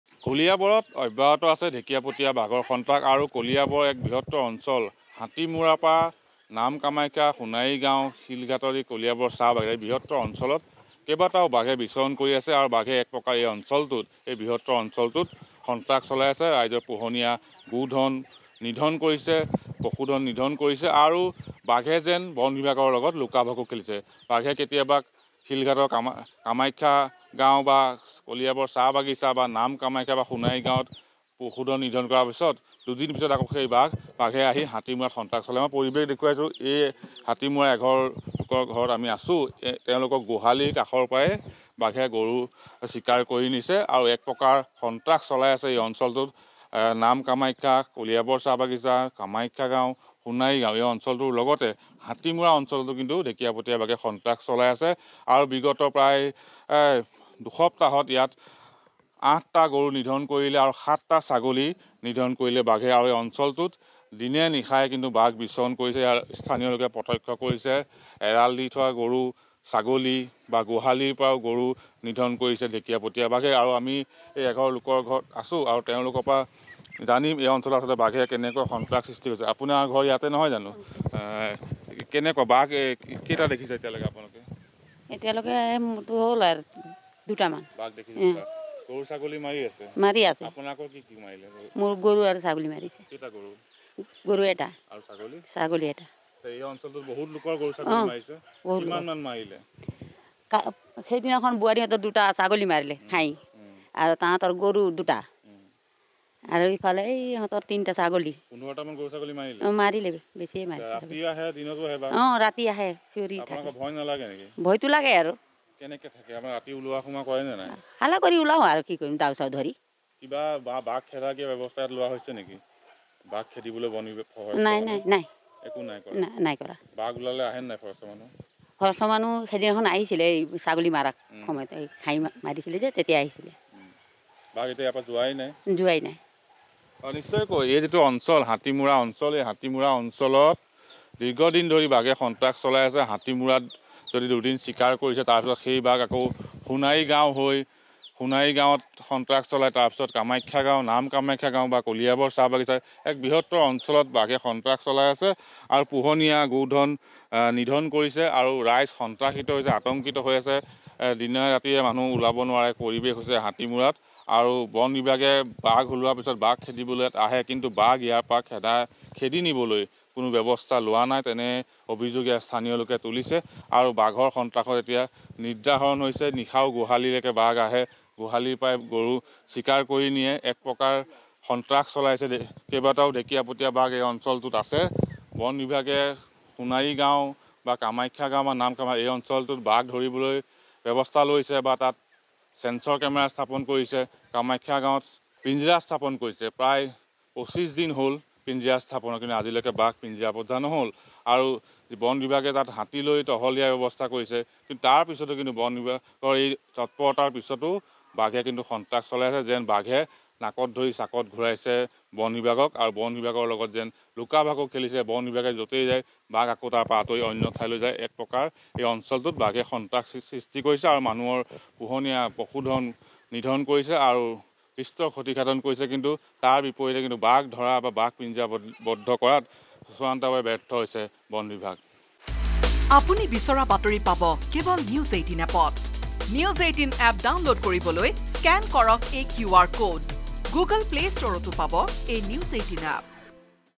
Kaliabor Tiger Terror : কলিয়াবৰত গাঁৱে গাঁৱে বিচৰণ বনৰজাৰ | N18V
Kaliabor Tiger Terror | কলিয়াবৰত গাঁৱে গাঁৱে বিচৰণ বনৰজাৰ। বিগত প্ৰায় এমাহে কলিয়াবৰ চাহ বাগিচা, হাতীমূৰা লগতে সোণাৰীগাঁও, কামাখ্যা গাঁও, নাম-কামাখ্যাৰ লগতে কলিয়াবৰ চাহ বাগিচাত বিচৰণ কৰি আছে তিনিটাকৈ ঢেঁকীয়াপতীয়া বাঘে।